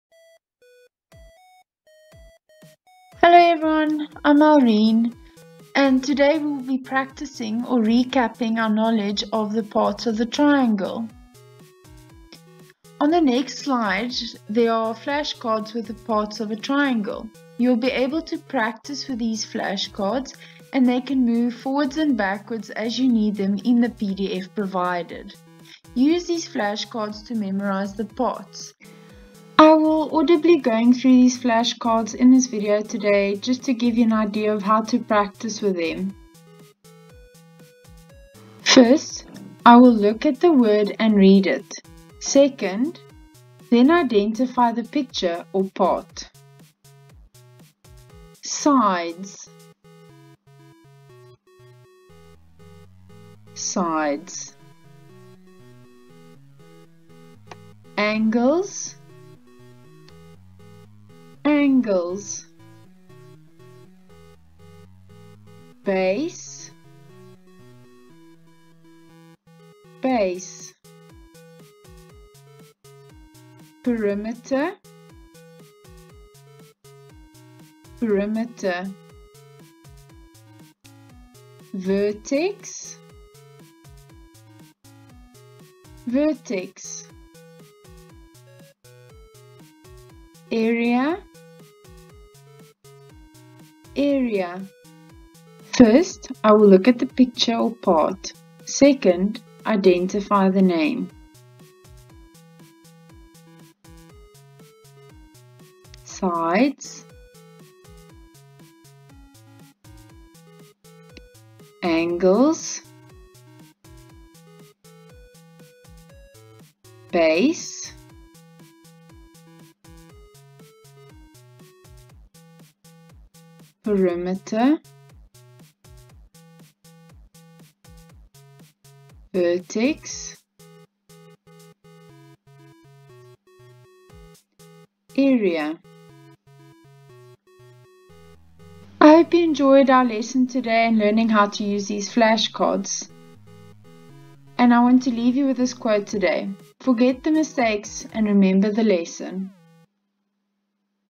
Parts of a Triangle Video Presentation
This is a Montessori inspired video explaining the different parts of a triangle.